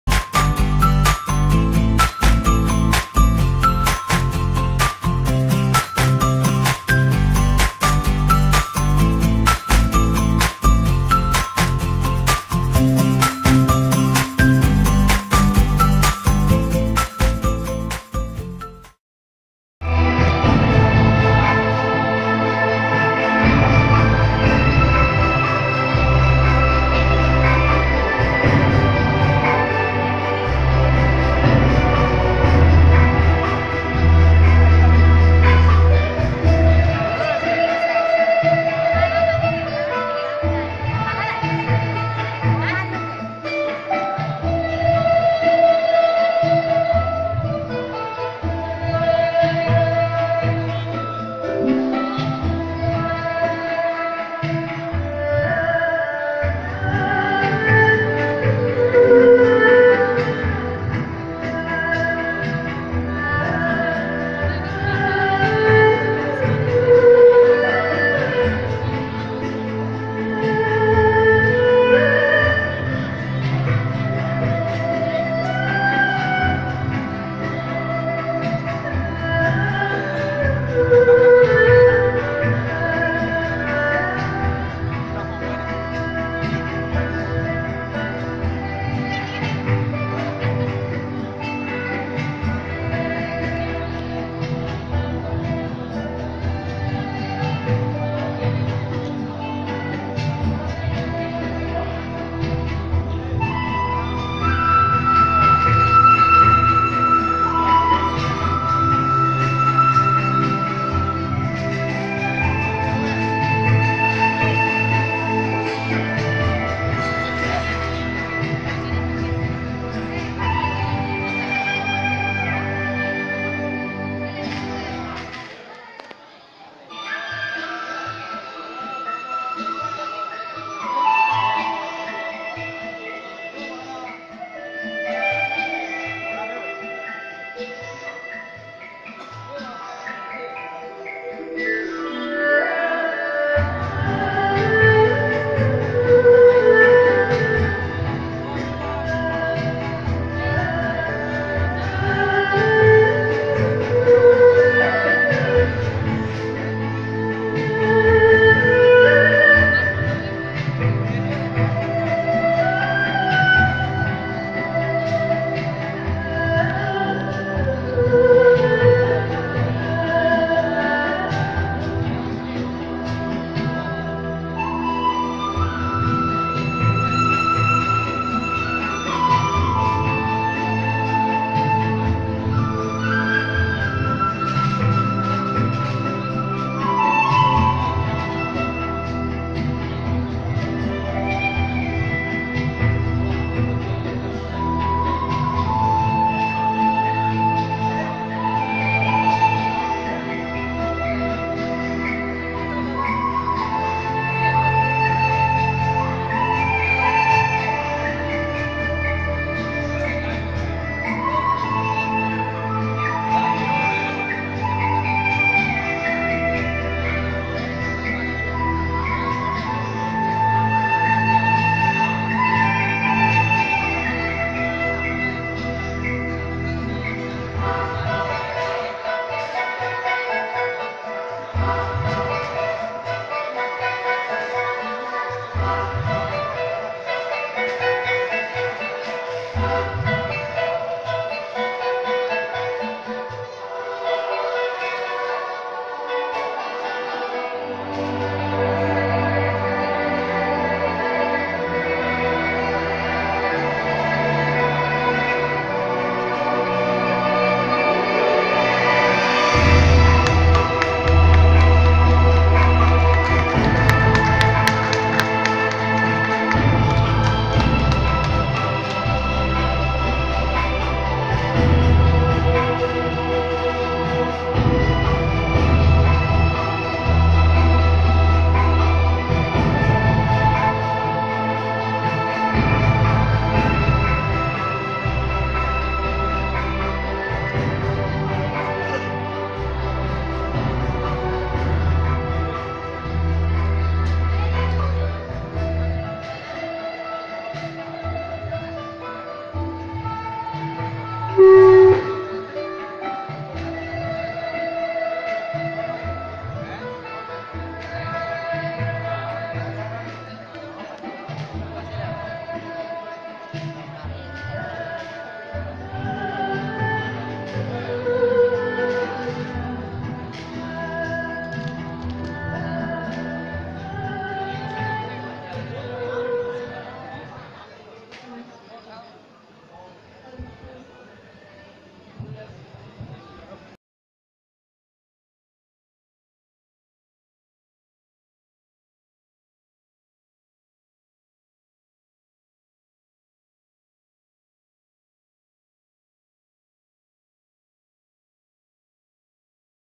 Mr. & Miss. Tai Ahom "Tai Ahom Cultural Group BOKOTA"- TAI AHOM CHANNEL
Tai Ahom Fashion Show.
"Tai Ahom Cultural Group BOKOTA"
Presents a Traditional TAI AHOM Beauty Pageant

(its Free)